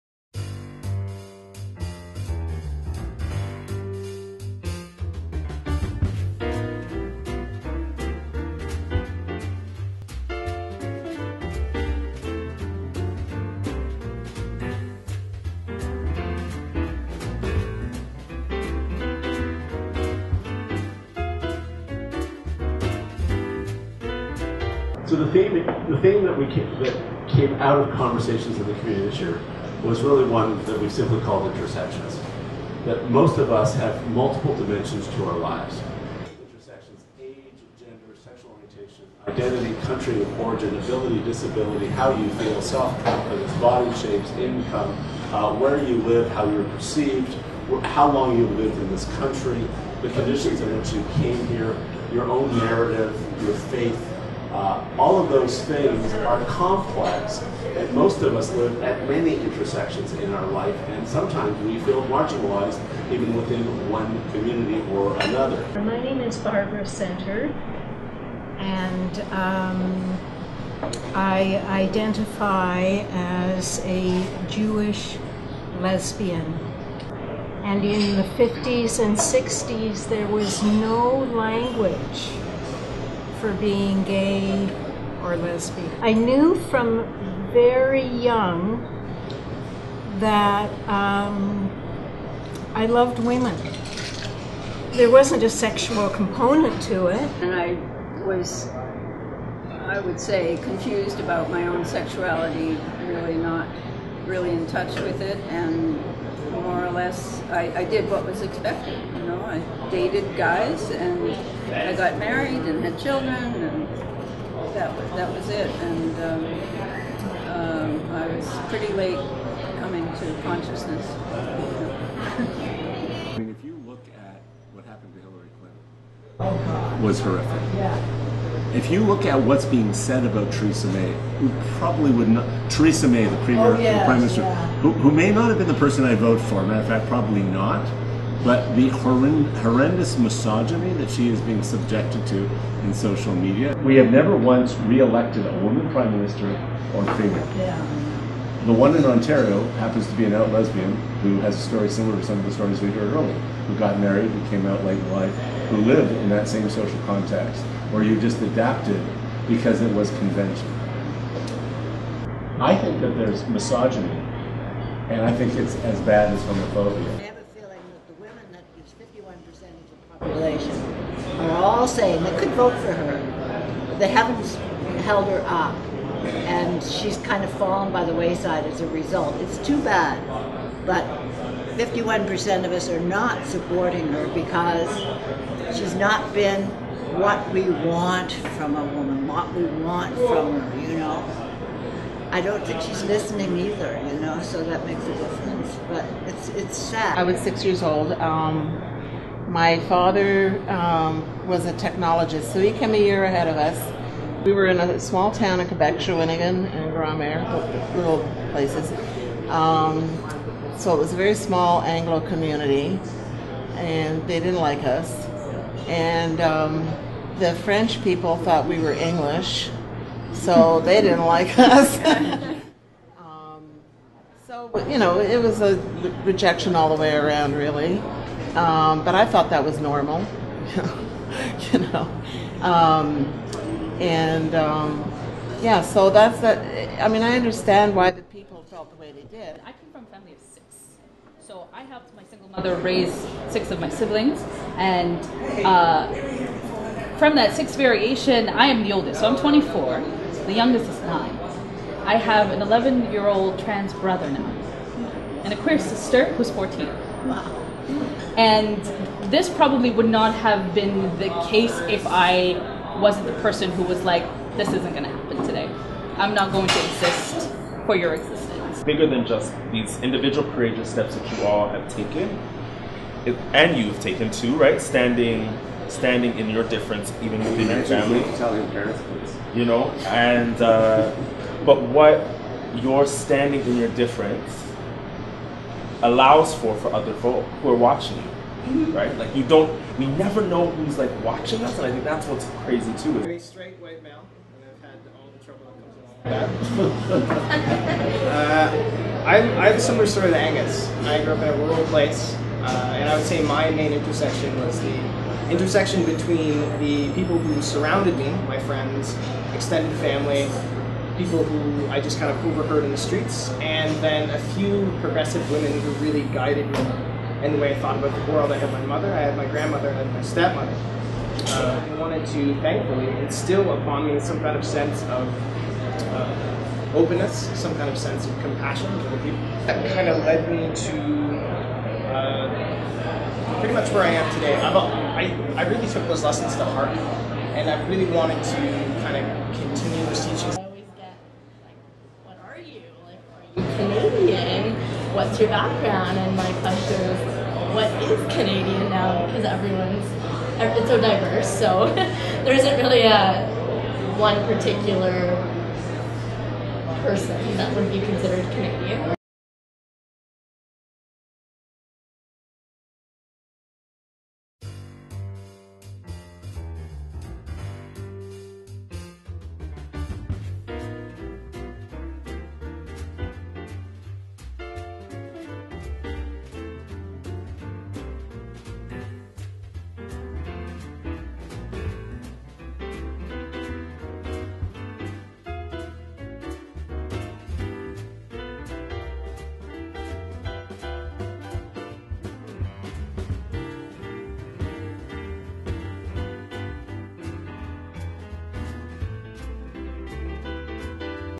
Idea Distillery
Idea Distillery is a conversation on intersectionality hosted by MPP Glen Murray. This segment, taking place on June 13 2017, is on gender and sexuality.